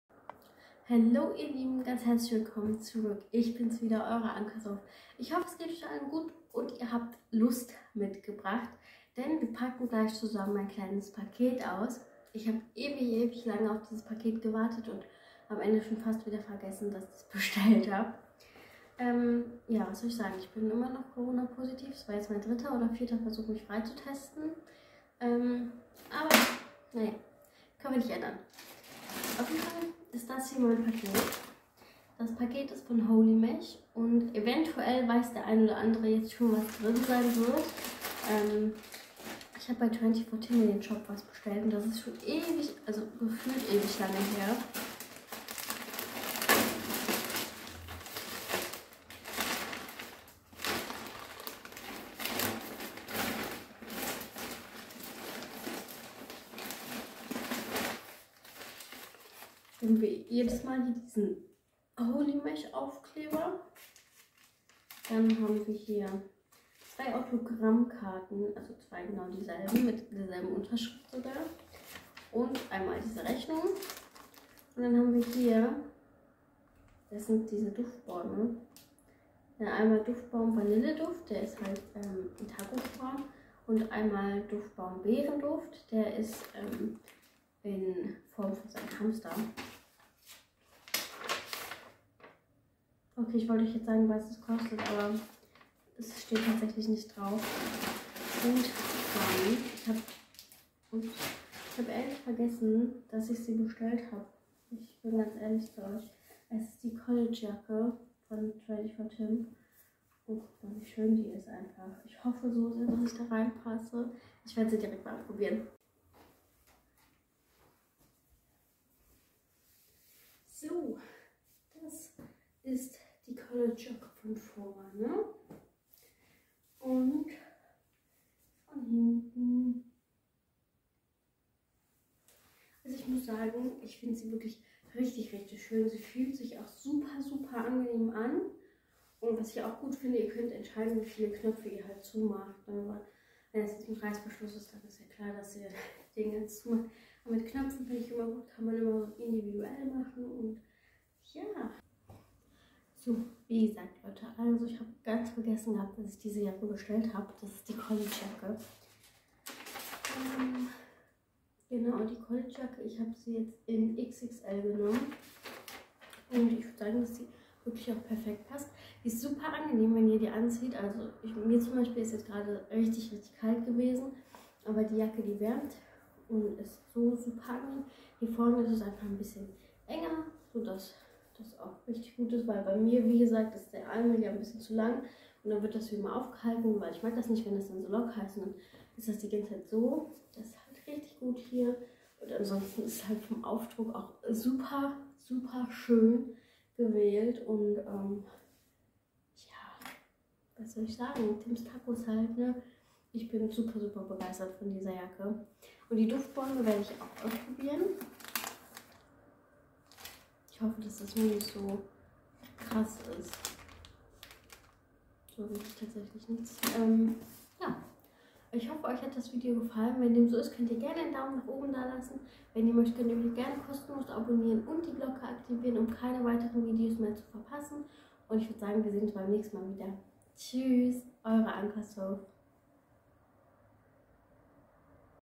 Unboxing! 🥳 Ich habe wieder im Shop von twenty4tim bestellt.
(Unbezahlte Werbung wegen Markenerkennung)
Hello und guten Morgen ihr Lieben ich wünsche euch allen einen wunderschönen Tag und hoffe es geht euch allen gut.

Hier ist noch ein Video aus meiner Quarantäne da mein anderes Video warum auch immer die ganze Zeit nicht hochgeladen werden kann gibt es jetzt erstmal das .

Wenn euch das Video gefällt würde ich mich sehr über einen 👍 freuen.